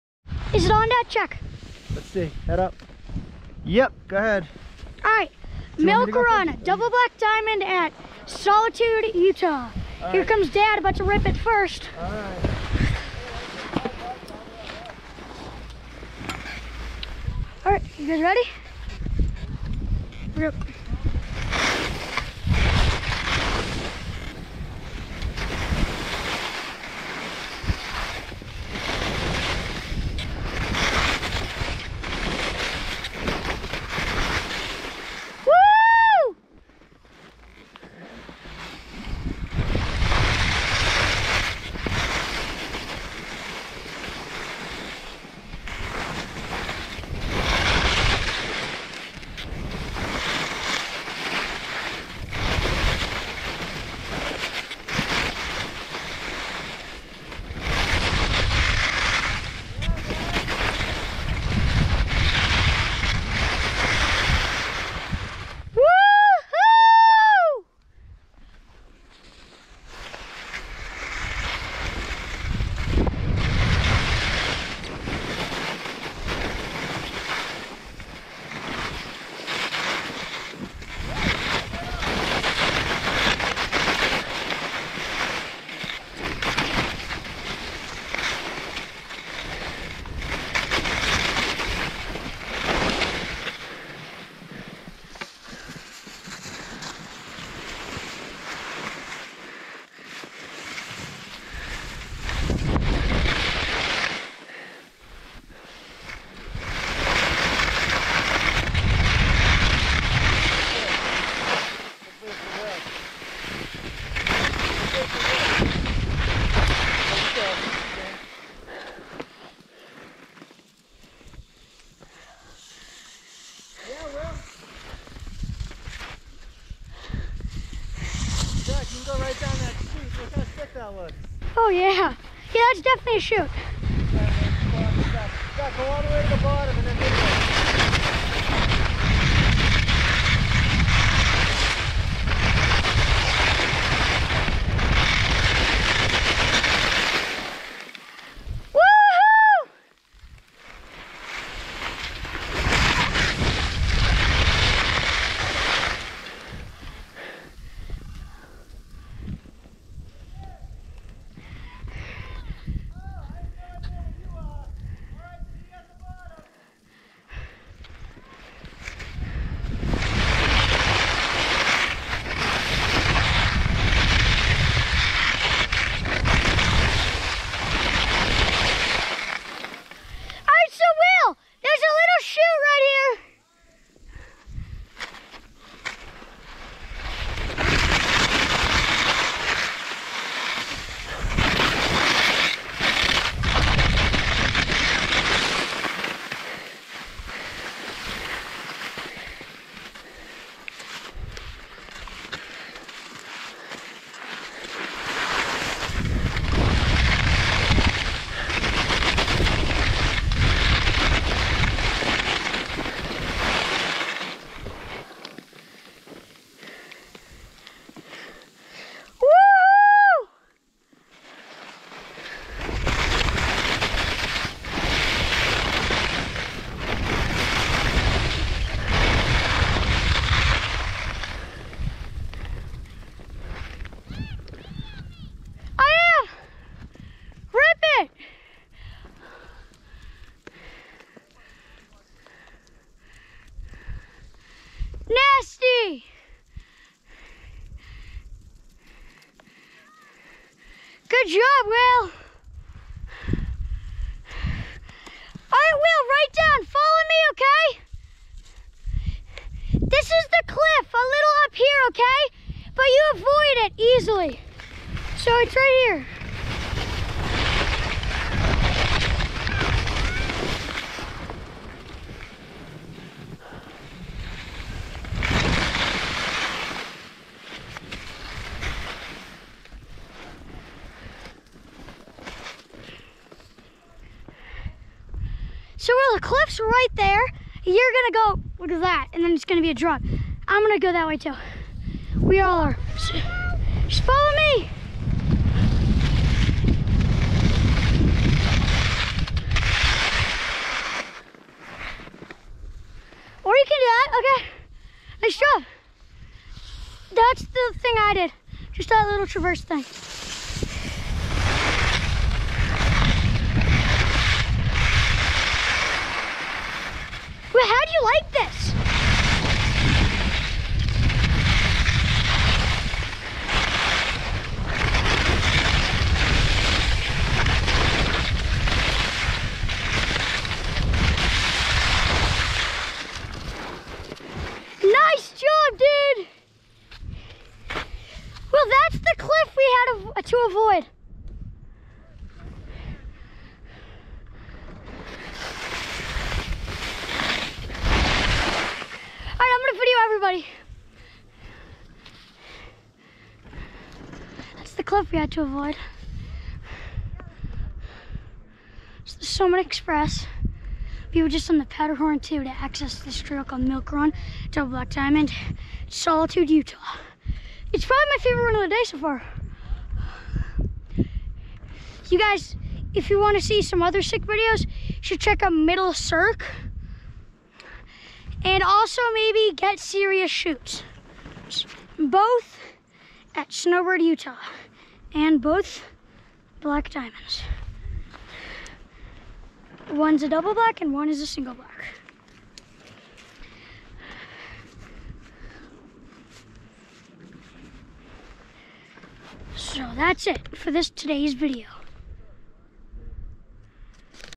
MILK RUN (Skiing) Solitude, Utah
This video is about one of the steepest ski trails in all of Utah!